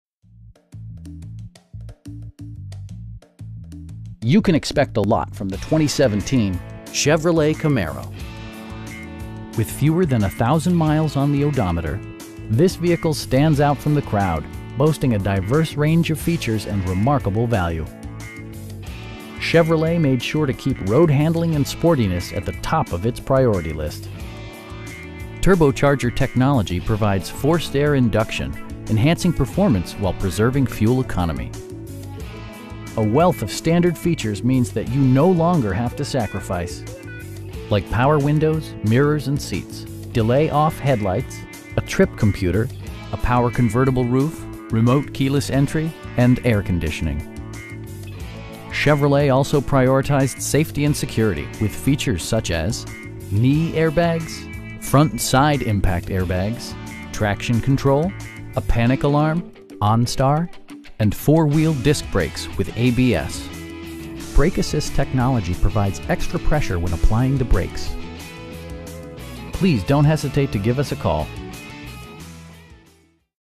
2017 Chevrolet Camaro 1LT in Saint Louis, MO 63143
Autohaus BMW
3015 S. Hanley Rd

You can expect a lot from the 2017 Chevrolet Camaro. With fewer than a thousand miles on the odometer, this vehicle stands out from the crowd, boasting a diverse range of features and remarkable value! Chevrolet made sure to keep road-handling and sportiness at the top of it's priority list. Turbocharger technology provides forced air induction, enhancing performance while preserving fuel economy. A wealth of standard features means that you no longer have to sacrifice. Like power windows, mirrors, and seats, delay-off headlights, a trip computer, front bucket seats, a power convertible roof, remote keyless entry, cruise control, and air conditioning. Chevrolet also prioritized safety and security with features such as: knee airbags, front side impact airbags, traction control, a panic alarm, onStar, and 4 wheel disc brakes with ABS. Brake assist technology provides extra pressure when applying the brakes. We pride ourselves in the quality that we offer on all of our vehicles. Please don't hesitate to give us a call.